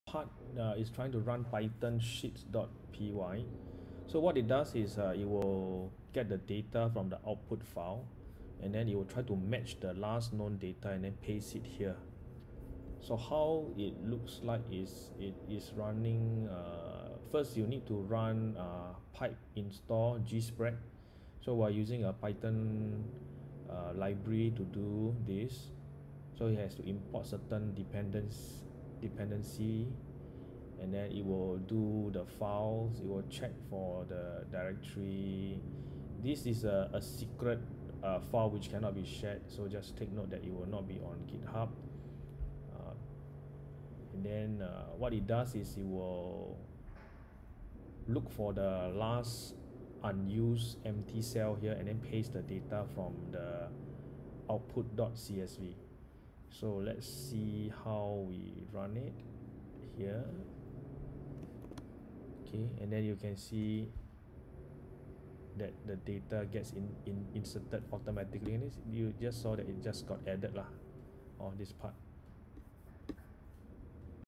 SLS how to use Python to update a Google Sheet by looking for an empty cell to paste data
SLS how to use Python to update a Google Sheet by looking for an empty cell to paste data from output.csv